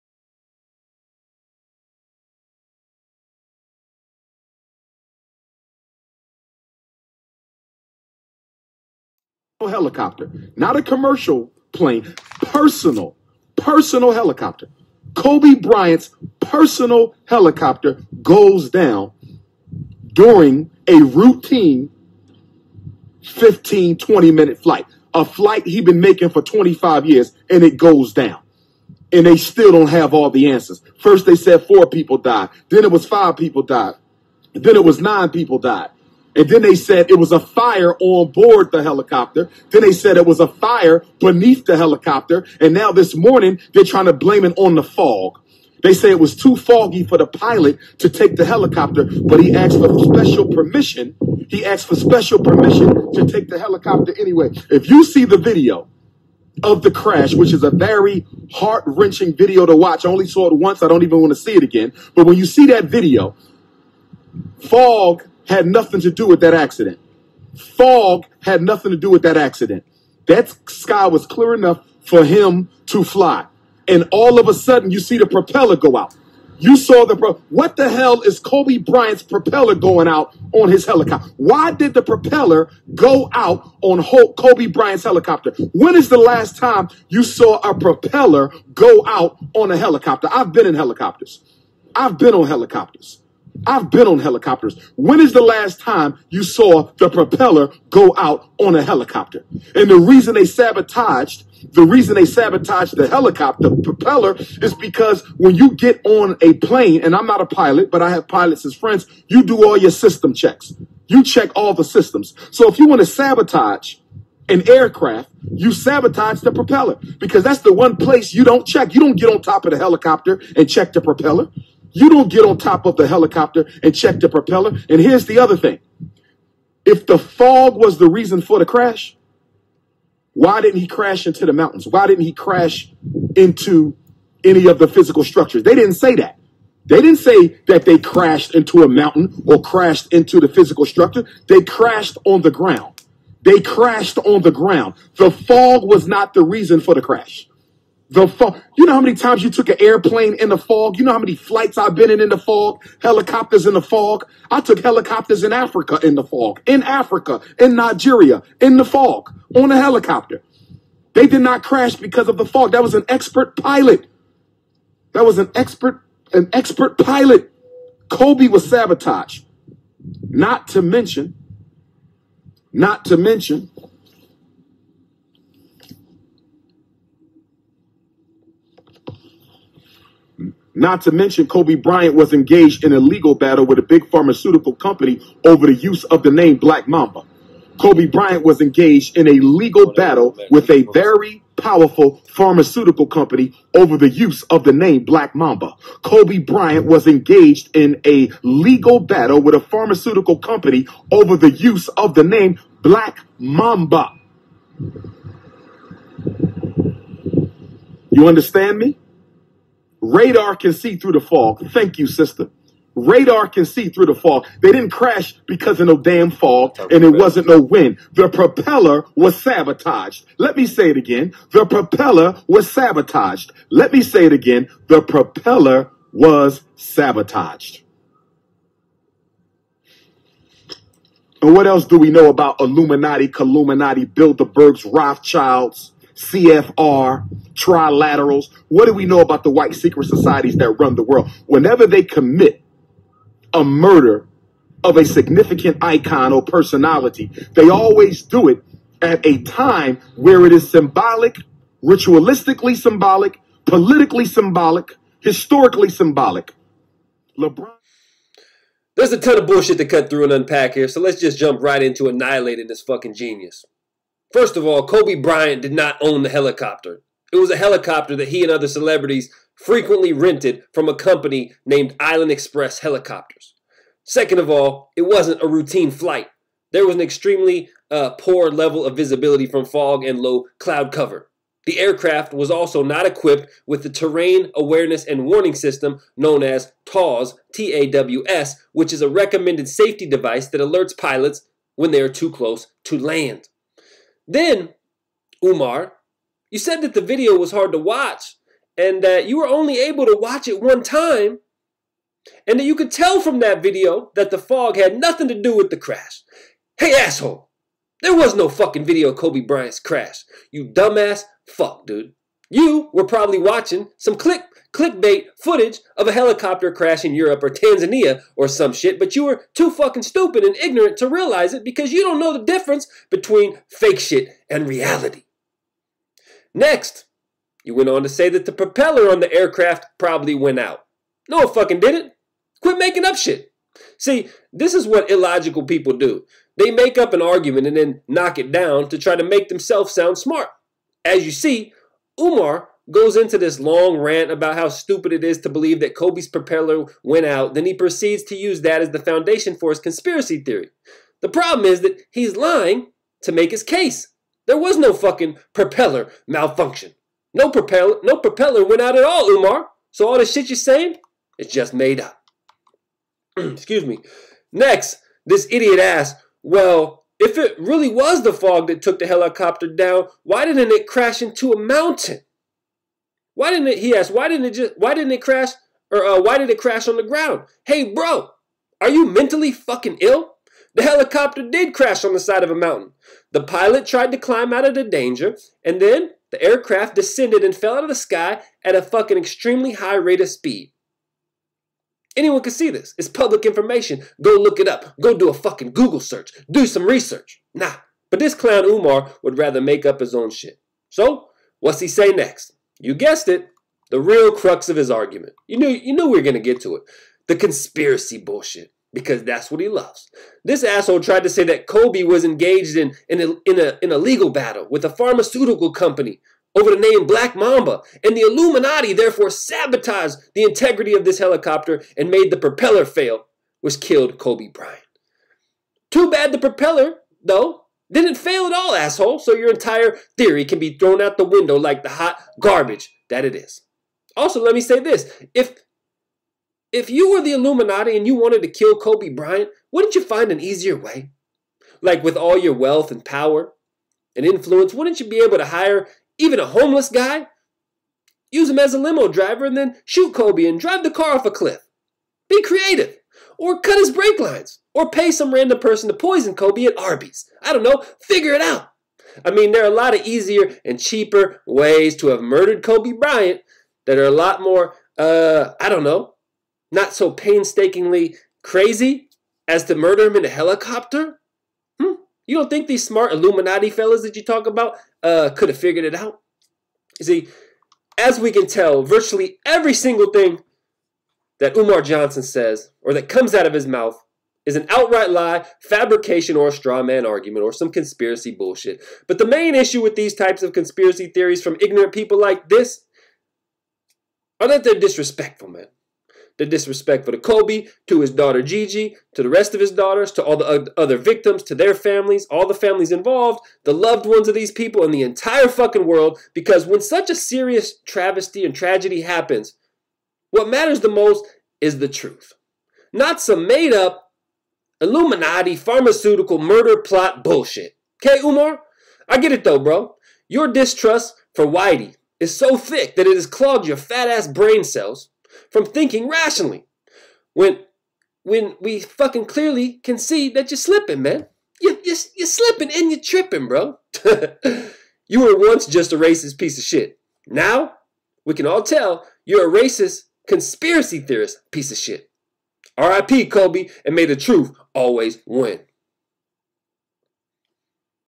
DAOTW: Dr. Umar Johnson
For DAOTW, Chad talks about Dr. Umar Johnson and his Kobe Bryant conspiracy theories. Don't forget to listen to the latest Logically Offensive podcast!! Now playing on iTunes and all your favorite podcasting platforms.